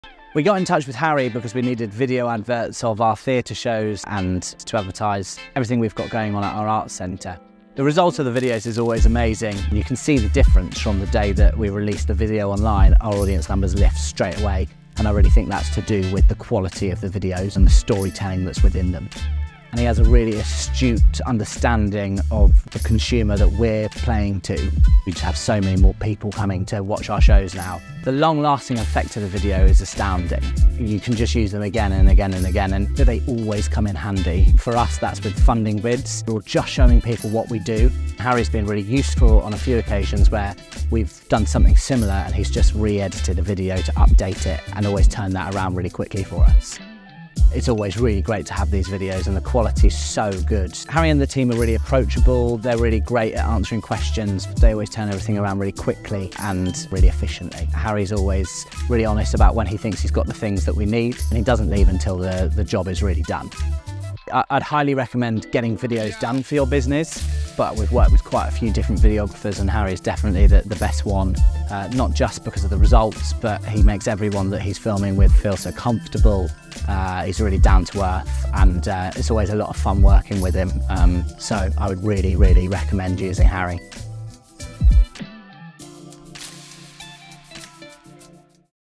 What Is It Like Working With Me? Client Testimonial Video: Arts Centre Hounslow
Check out this client testimonial from Jonathan Ashby-Rock, Co-Founder and Artistic Director of The Arts Centre Hounslow!